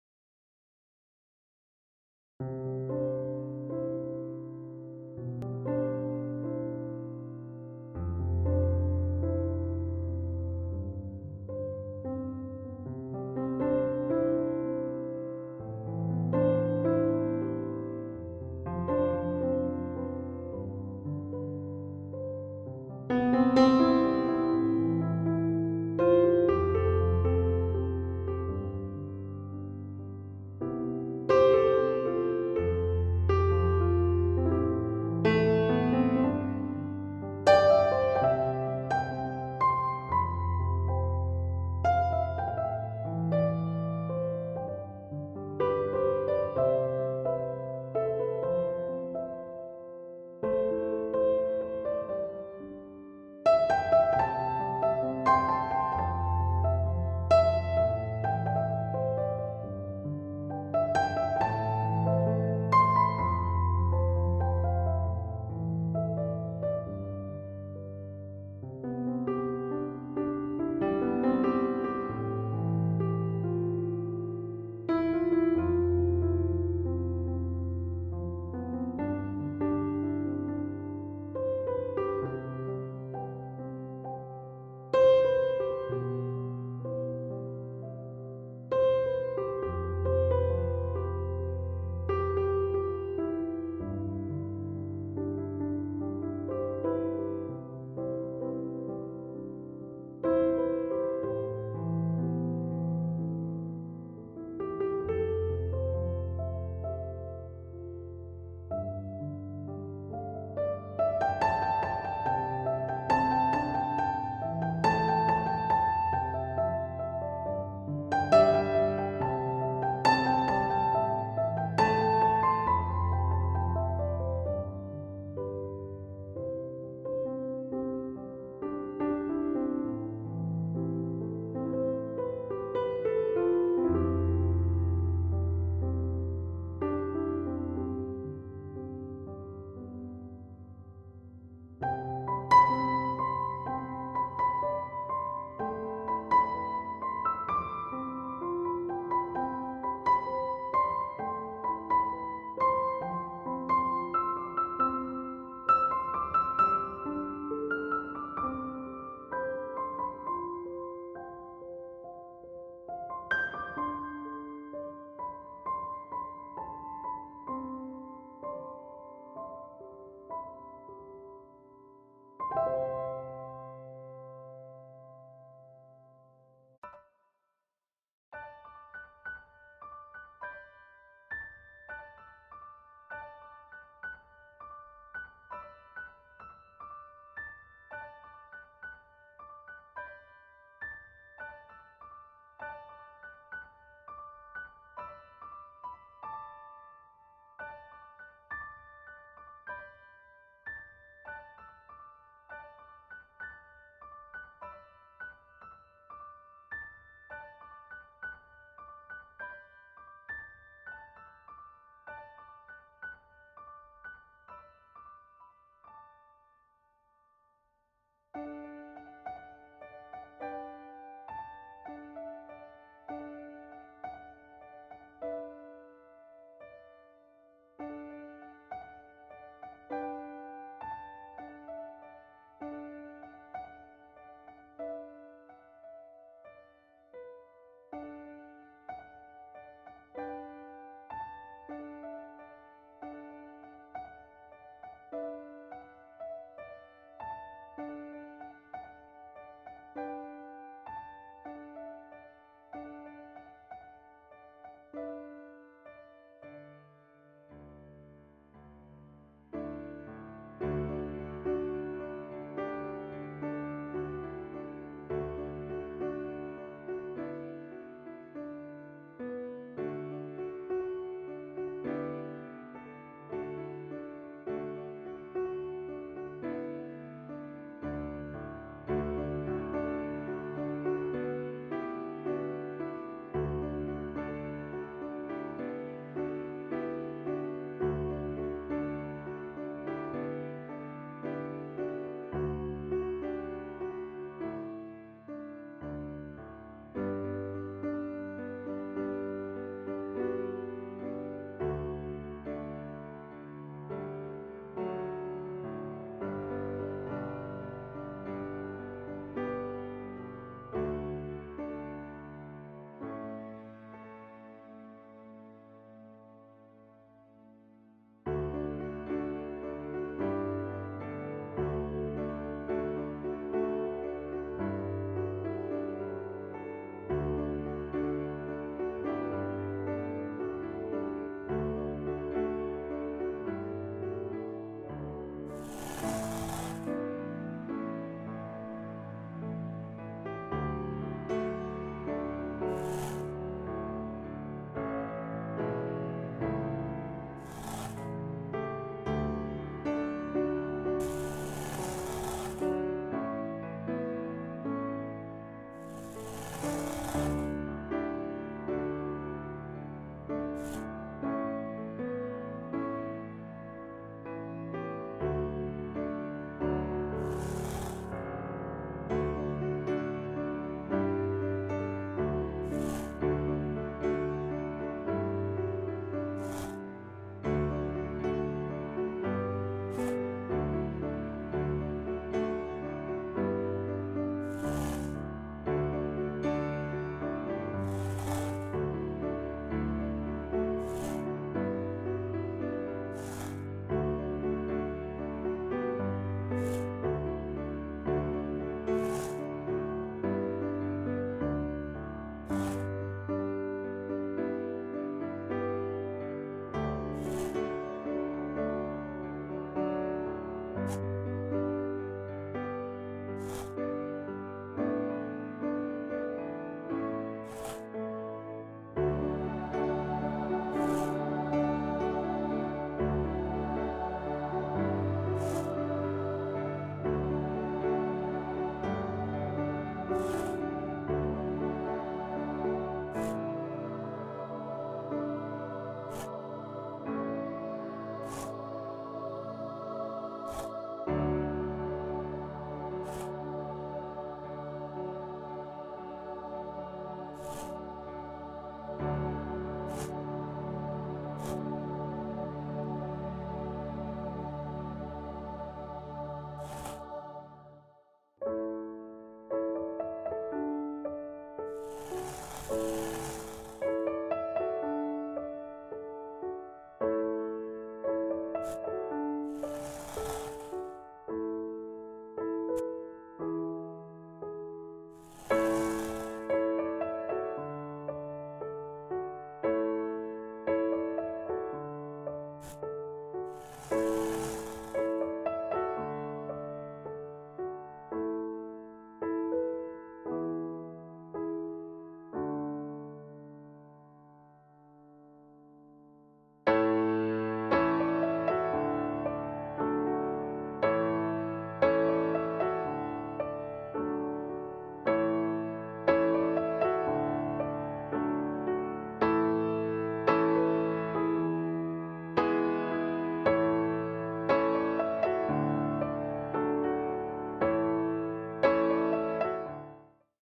Geometric Bear / Tape Art / Masking Tape Painting ideas #27 / Satisfying Asmr
Hello :-)
I painted Geometric Bear using masking tape 🎨